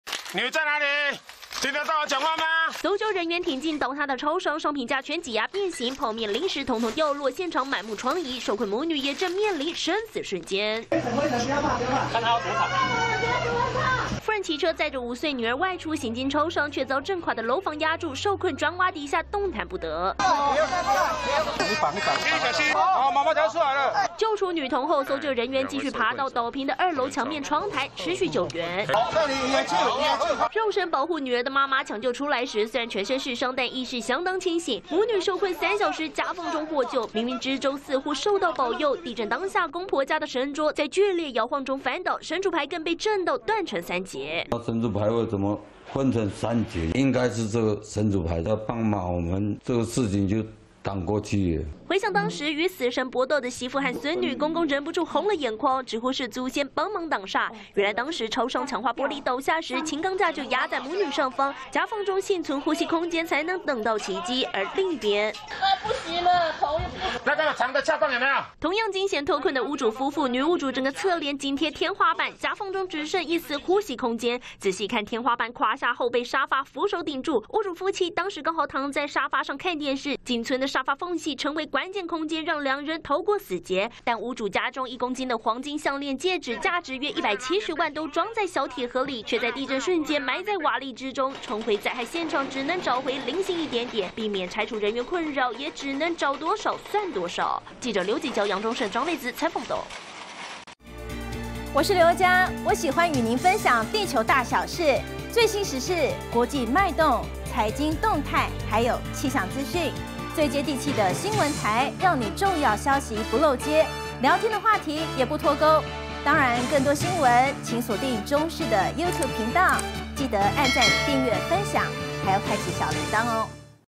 母女受困崩塌超商獲救 公公:神主牌"斷三截"擋煞│中視新聞 20220920
花蓮玉里鎮超商建築震垮倒塌，母女被樓房壓三小時，驚險獲救，事後受困媽媽的公公透漏，地震當下天搖地動，家中神主牌掉在地上斷成三截，直呼是祖先"擋煞"，而倒塌屋主夫妻雖然幸運生還，但房屋被震倒，約有1公斤左右的黃金，埋在廢墟內，今天返回現場挖找，只找到零星的金飾。